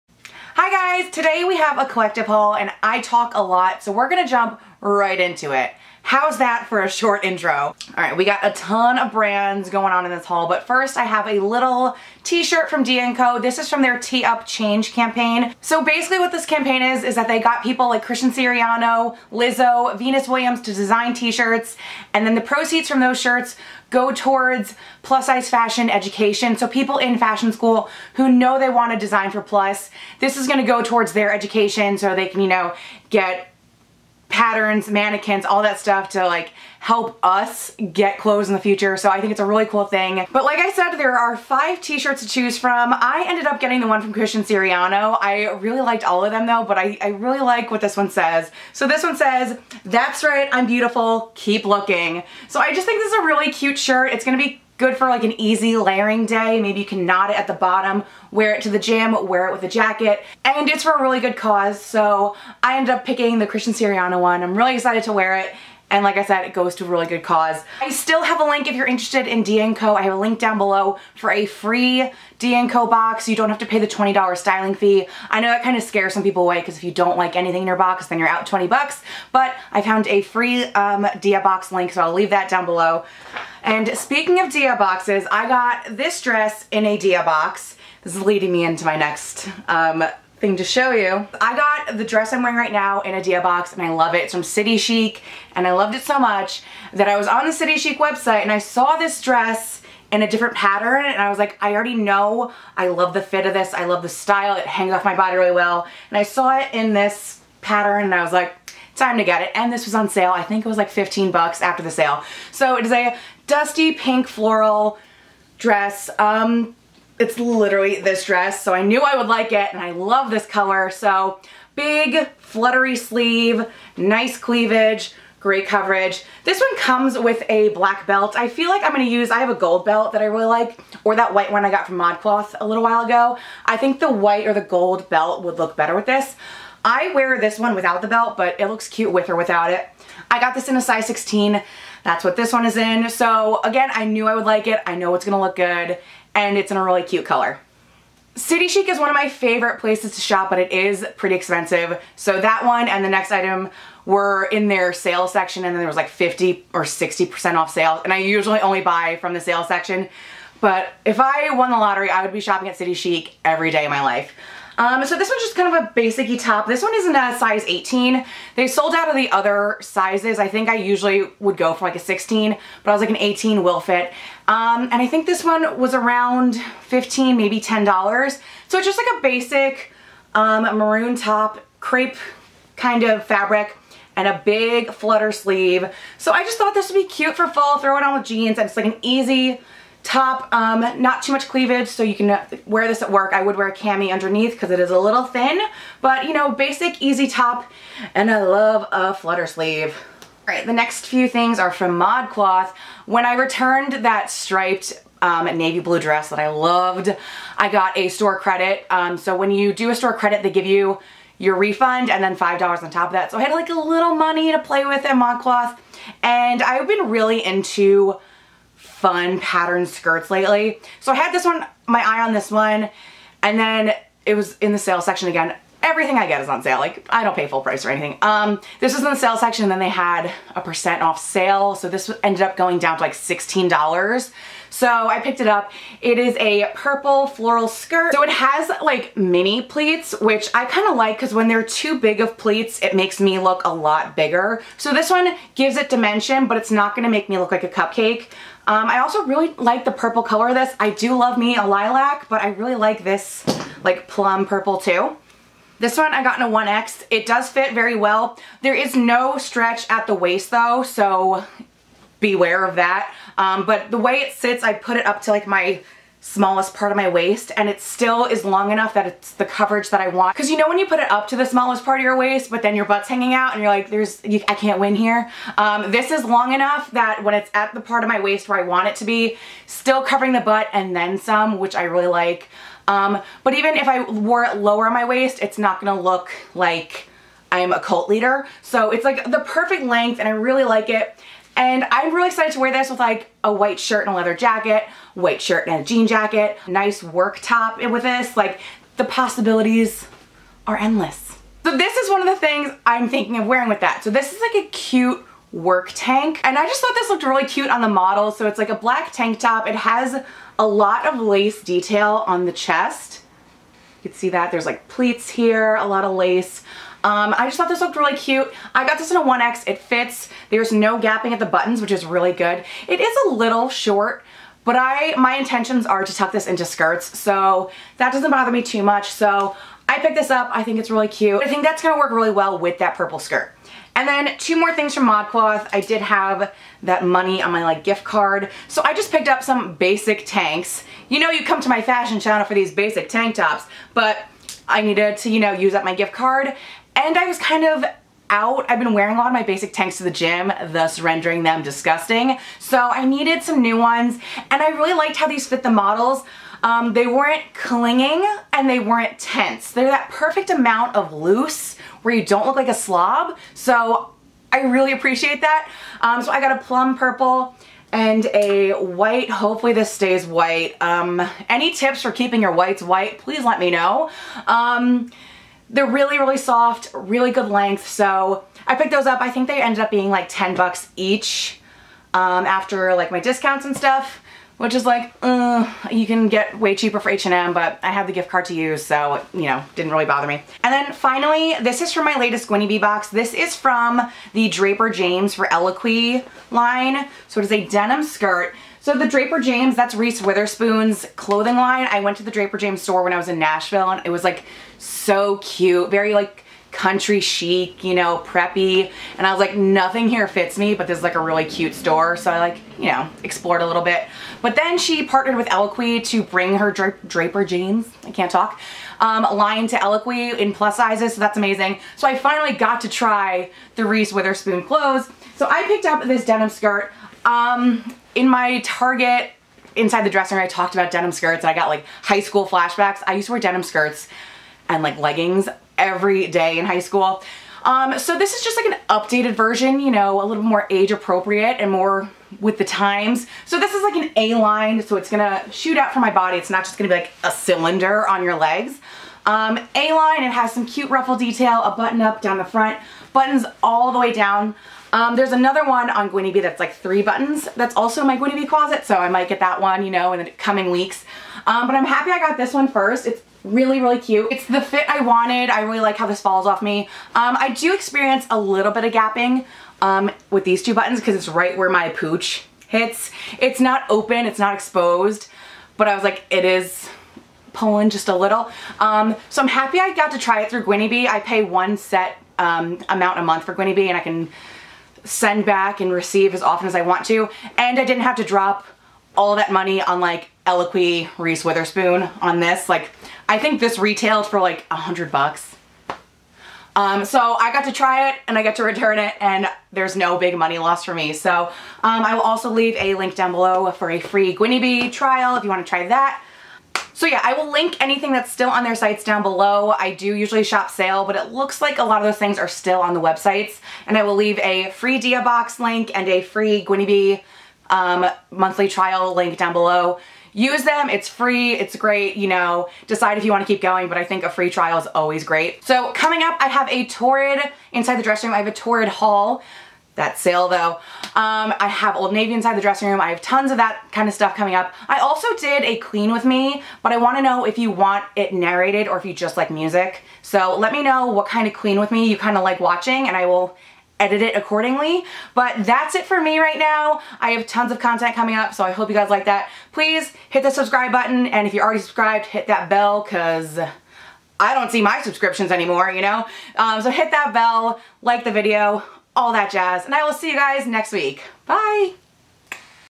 Plus Size TRY ON Collective Haul | Dia&Co, City Chic, Modcloth & Eloquii | FALL 2018
*Free Styling Fee is for New Dia users*

City Chic Pink Dress (Size 16) (Currently on Sale!!!)
Bell Sleeve Top (Size 18)
Purple Floral Pleated Skirt (1X)
Black Lace Tank (1X)
Purple Tank (1X)
White Tanks (1X)
Eloquii Skirt (Size 16) (ON SALE NOW!)

I'm on a book cover!!!